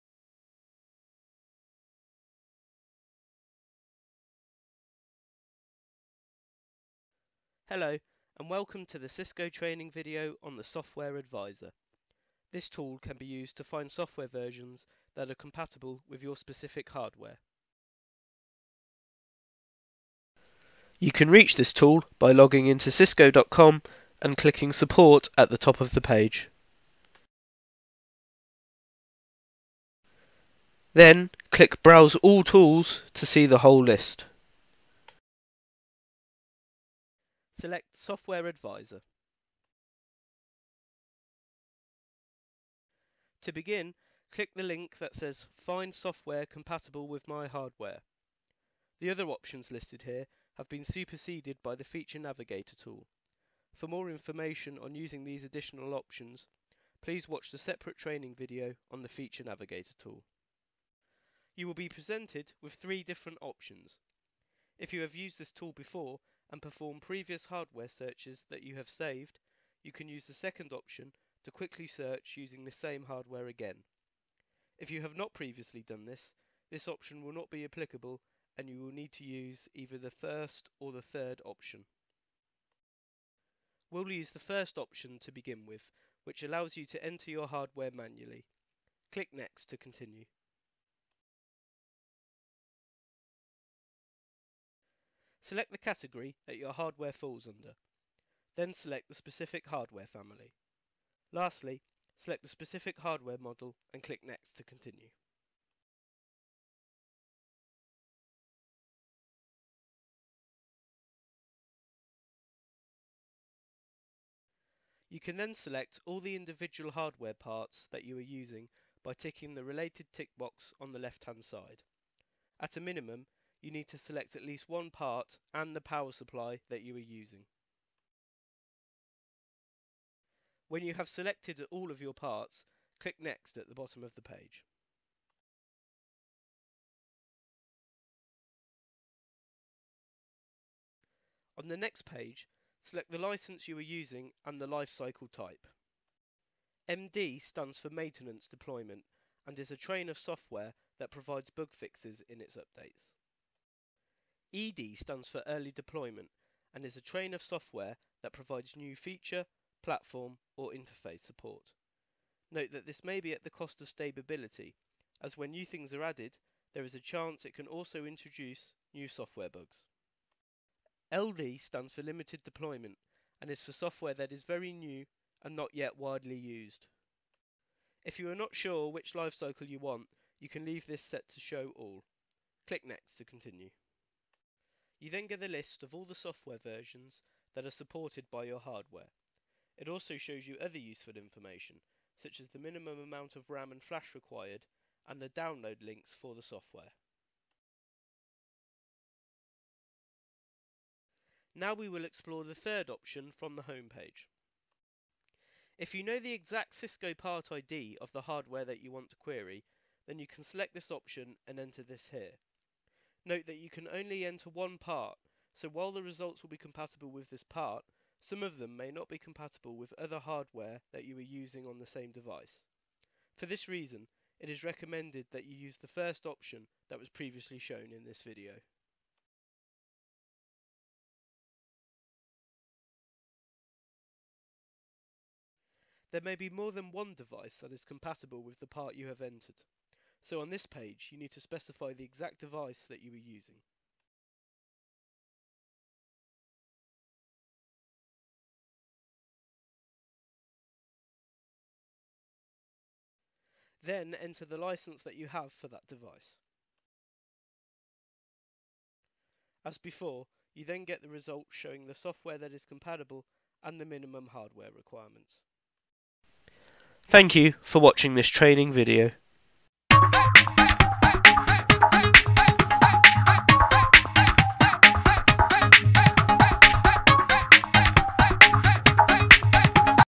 Software Advisor Tool
Training Video on how to use the Software Advisor tool to find software versions that are compatible with specific Cisco hardware.